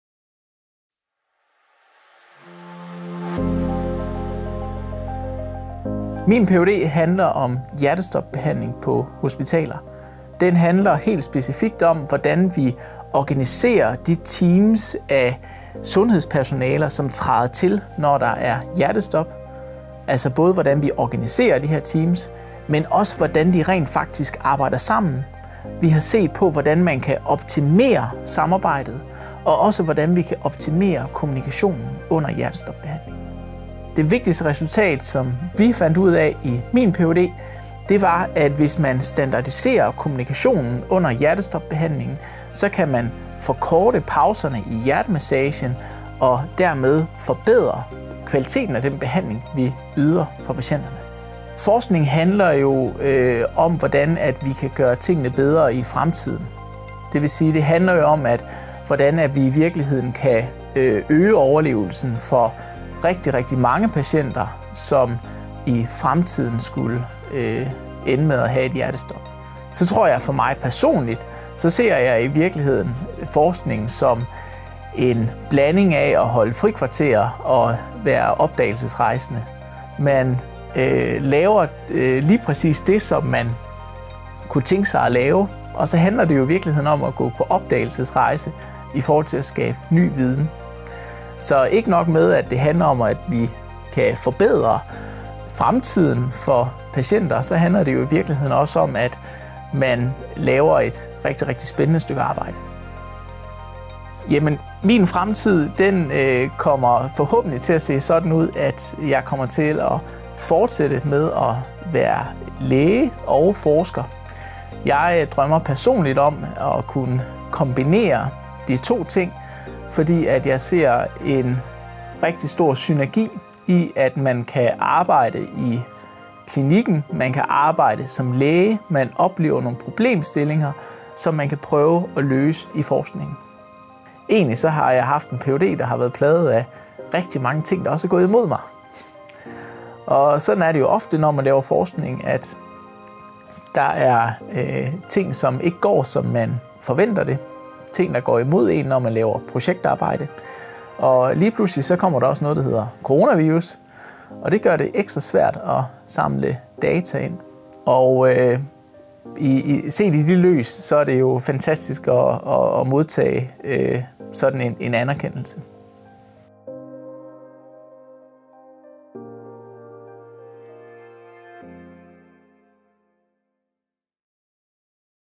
Ph.d.-prisvinder 2022: Kasper Glerup Lauridsen, Hjertestop på hospitaler
Læge Kasper Glerup Lau- ridsen har vist, hvordan behandling af hjertestop hos indlagte patienter ofte kan forbedres væsentligt ved at minimere pauserne i hjertemassagen.
Nedtællingen fra teamlederen kommer roligt og forudsigeligt trods den uventede situation med en ung patient, der lige nu modtager hjertemassage:
”Rytmetjek om femten sekunder. Vi skal også have tjekket pulsen. Alle klar! Fem-fire-tre-to-én. Stop hjertemassagen.”
Hjertestopholdets medlemmer ken- der hver deres vigtige funktion, og de kender replikkerne: ”Ingen puls”, lyder det efter få sekunder fra sygeplejersken samtidig med, at lægen via sin monitor må konstatere, at der heller ikke er en stødbar hjerterytme, så defibrillatoren skal endnu ikke bruges. ”Start hjerte- massagen,” lyder kommandoen derfor.
Det er ph.d. Kasper Glerup Lauridsen, som skitserer ovenstående scene. I sin afhandling viser han, hvordan virkeligheden på et hospital dog kan se noget anderledes ud. I praksis medfører fraværet af effektive rutiner og fælles sprog ofte en unødvendig lang pause i hjertemassagen, hvilket er forbundet med ringere chance for at overleve.